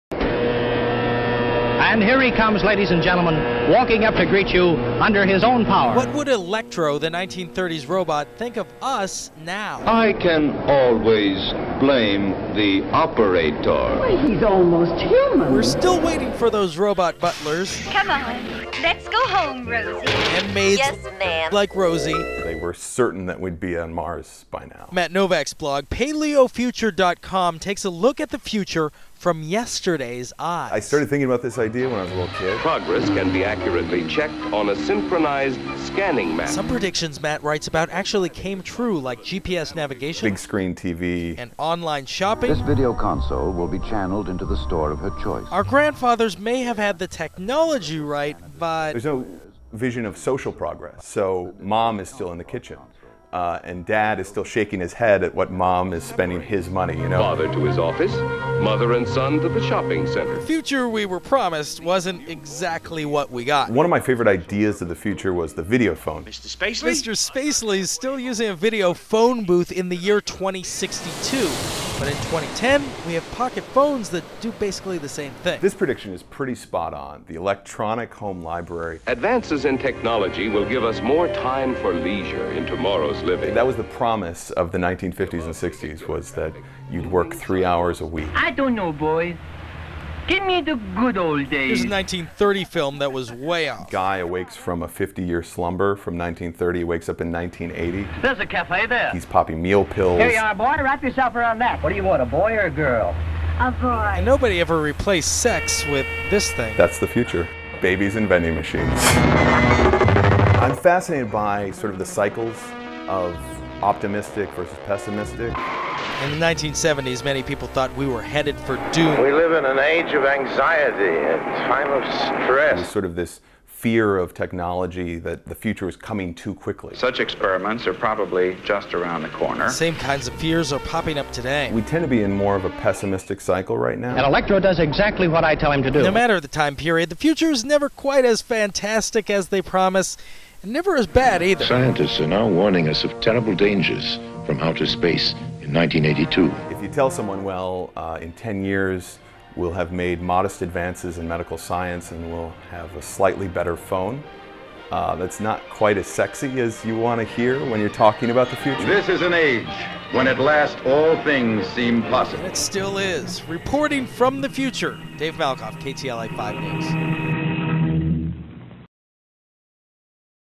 The Future of The Past
A look at what our grandparents thought the future would be like. Some predictions were spot-on, some were way off. Dave Malkoff ] reports from the future.